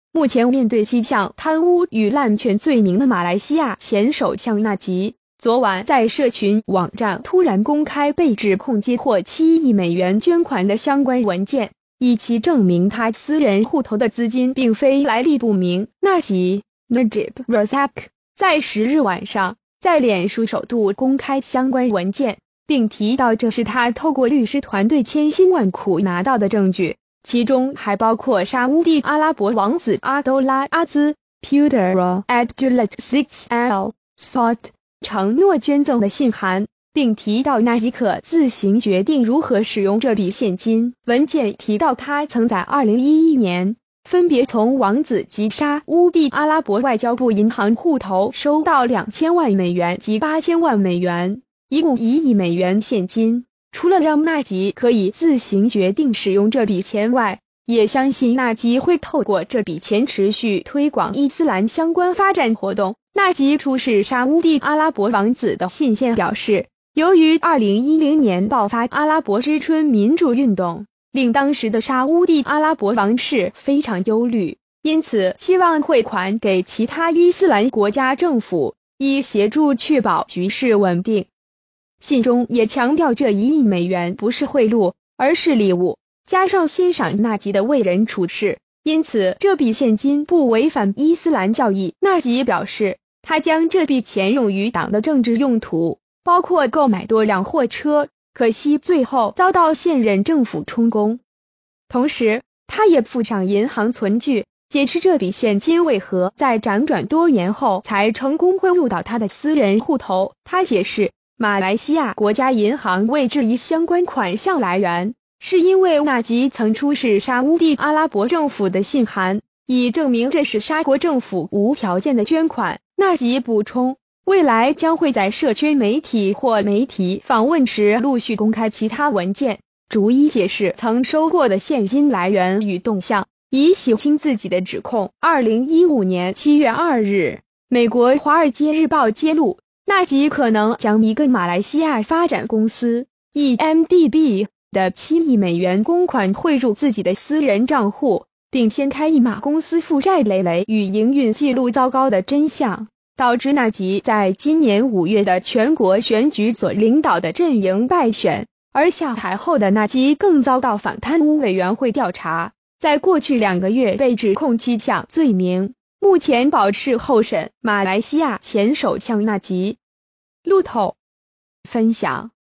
納吉臉書出示文件 解釋私人戶頭原由
目前面對7項貪污與濫權罪名的馬來西亞前首相納吉，昨晚在社群網站突然公開被指控接獲7億美元捐款的相關文件，以期證明他私人戶頭的資金並非來歷不明。

發表評論評論如下。